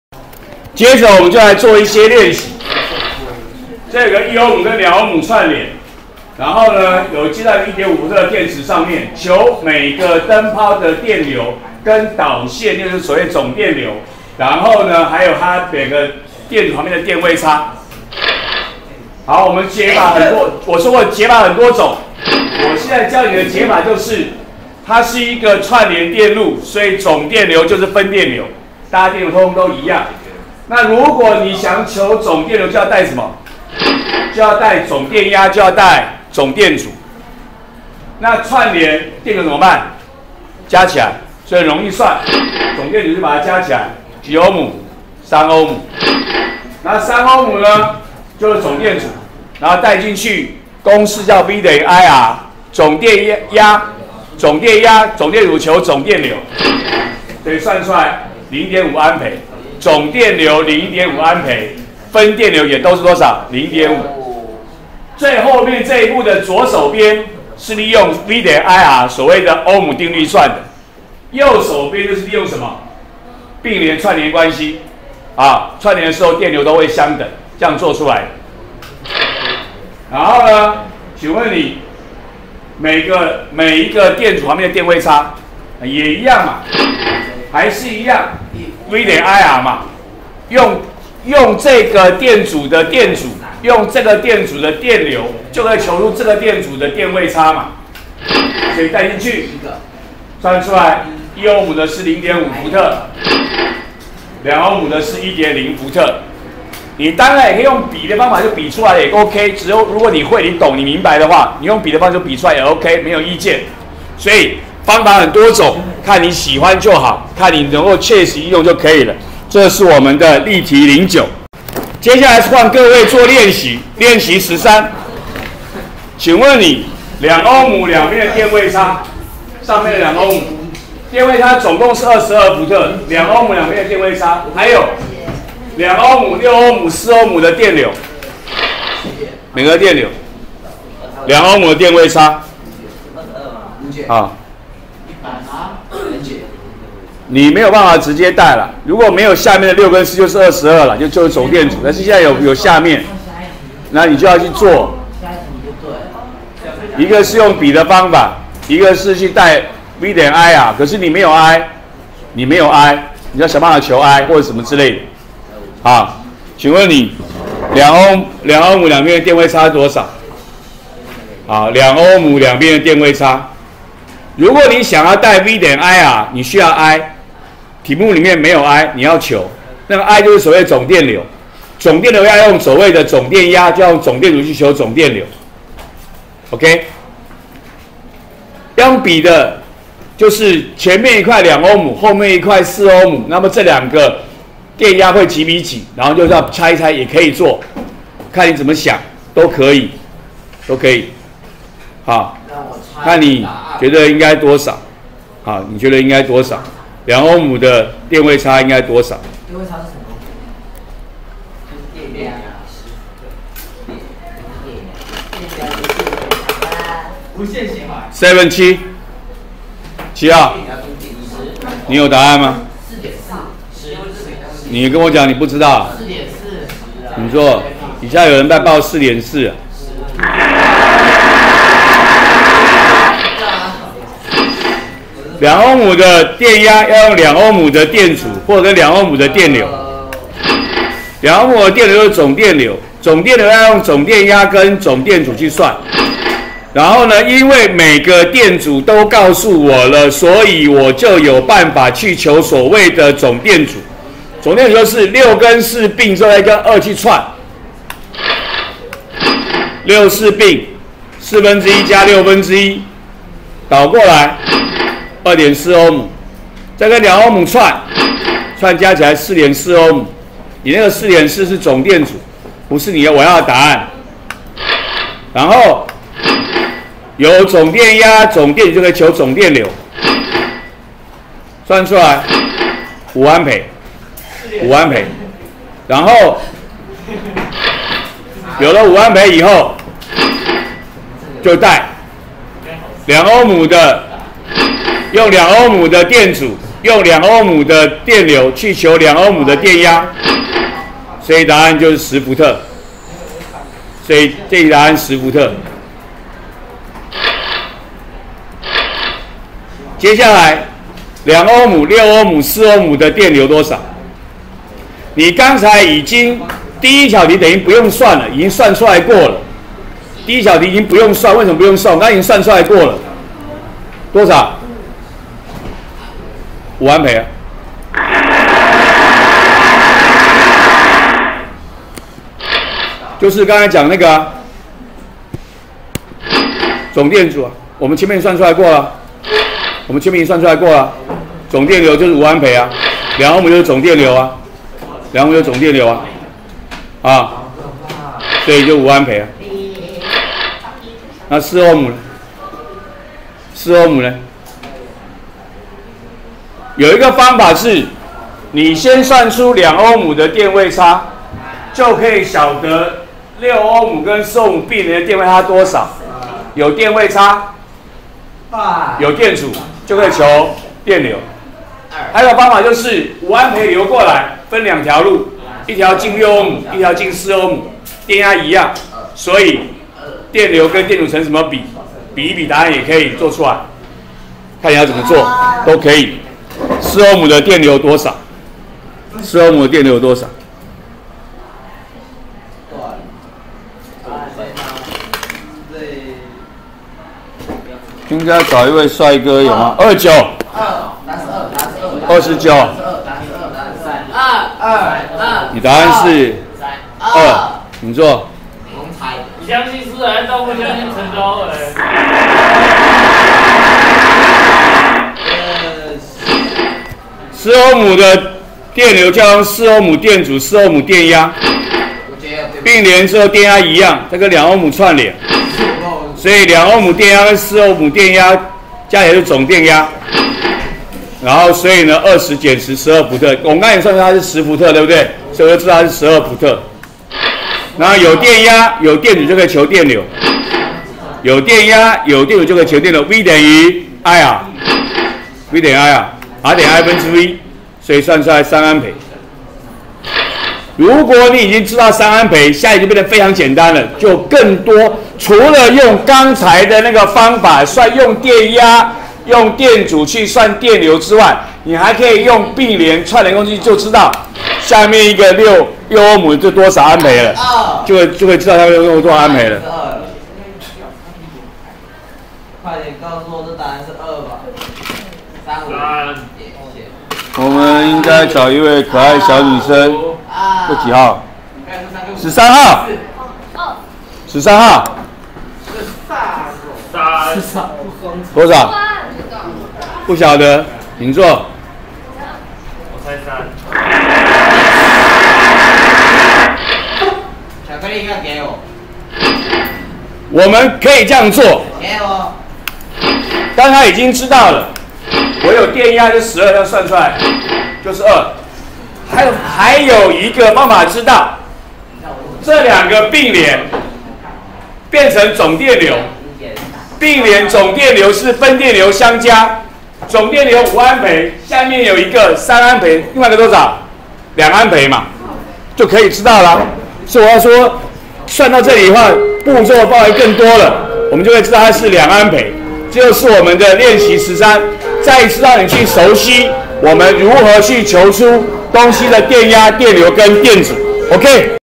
103學年國三理化 直流電路23：例題09、練習13
例題09、練習13都是給電路，電阻已知，電壓已知，例題09求總電流、各燈泡電流、各燈泡電壓，練習13求某電阻電壓，各電阻電流。